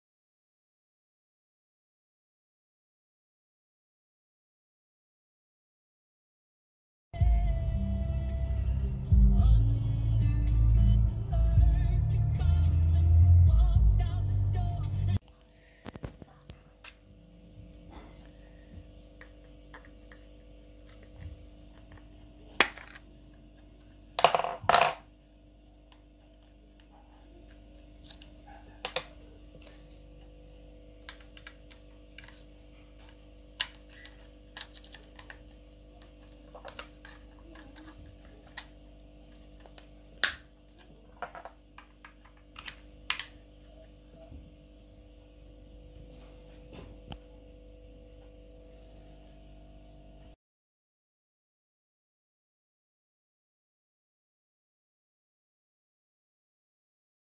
How to change Renault Key Card battery
Samuel, who is 11 years old demonstrate how to change battery in Renault Clio key card. It's very simple.